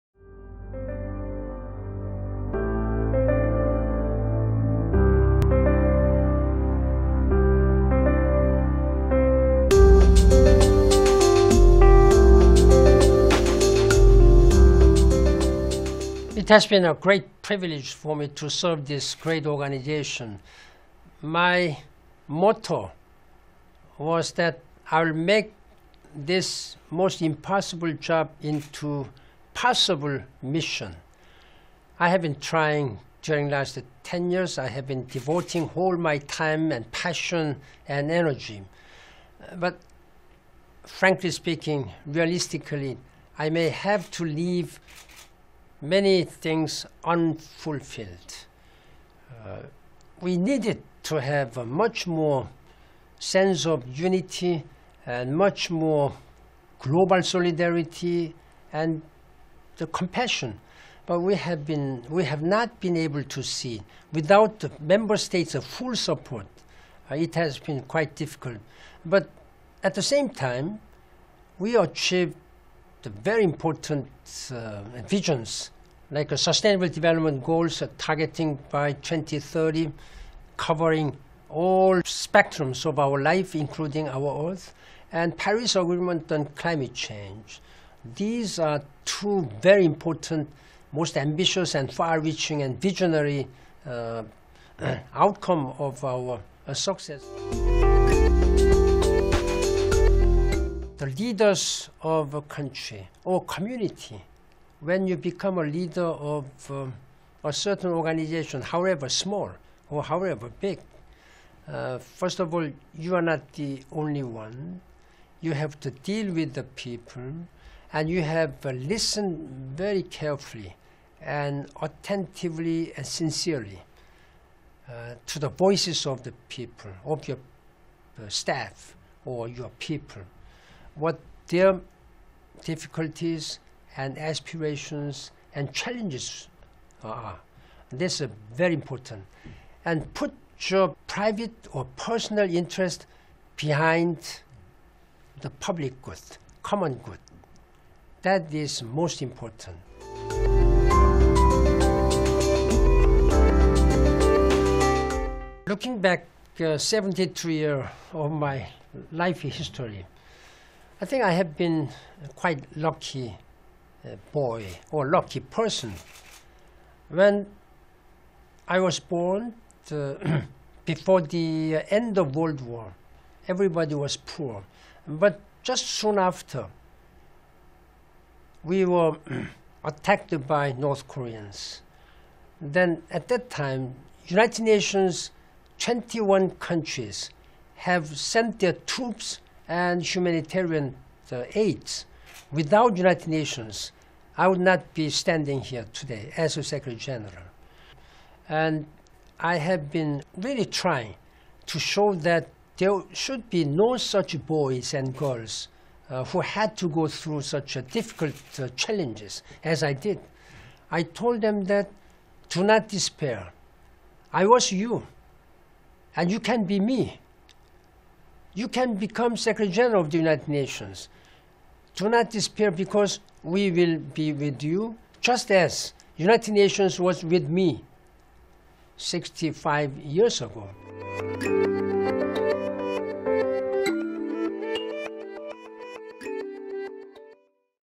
Ban Ki-moon quis transformar trabalho ‘impossível em missão possível’
Em sua última entrevista como secretário-geral da ONU, Ban Ki-moon fala sobre os desafios de um trabalho considerado impossível, relembra a juventude na Coreia do Sul, fala de ensinamentos de Confúcio e faz um balanço de seu legado na década em que esteve à frente das Nações Unidas.